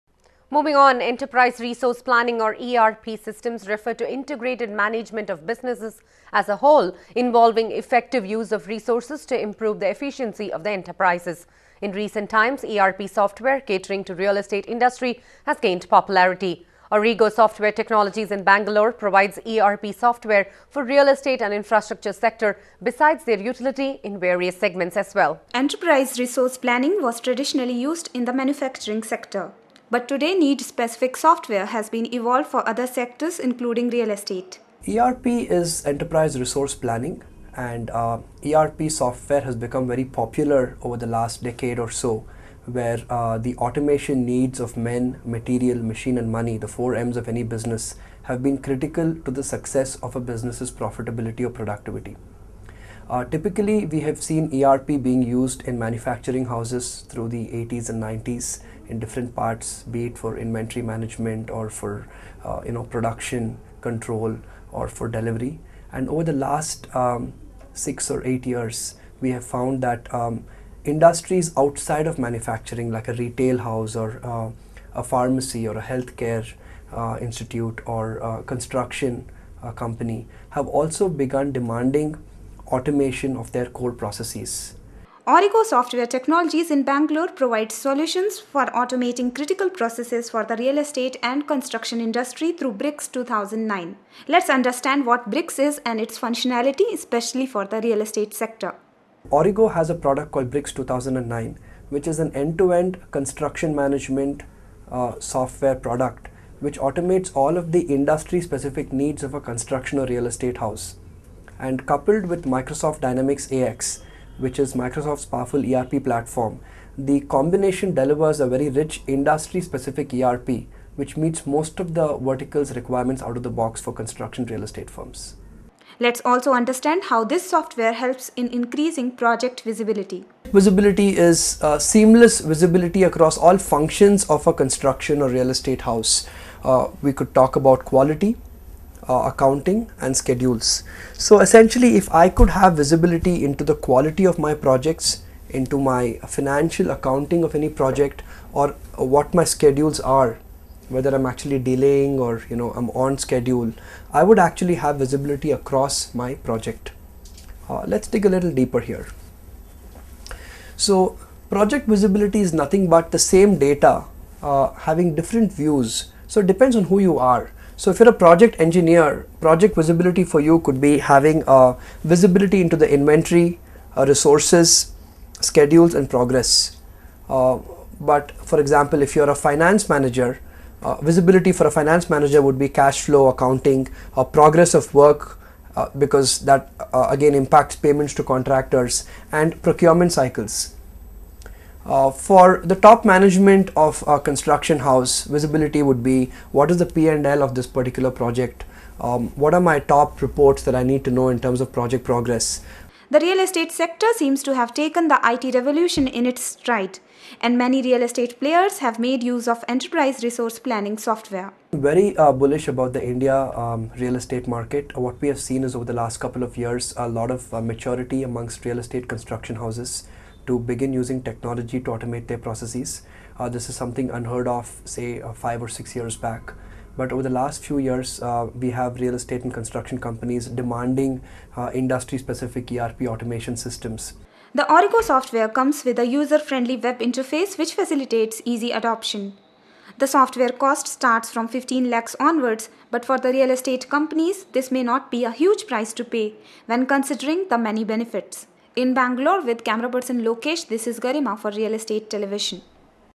Enterprise Resource Planning for Real Estate Sector
Enterprise Resource Planning or ERP systems refer to integrated management of businesses as a whole involving effective use of resources to improve the efficiency of the enterprises. In recent times ERP software catering to real estate industry have gained popularity. Aurigo Software Technologies in Bangalore provides ERP software for real estate and infrastructure sector besides their utility in various segments as well.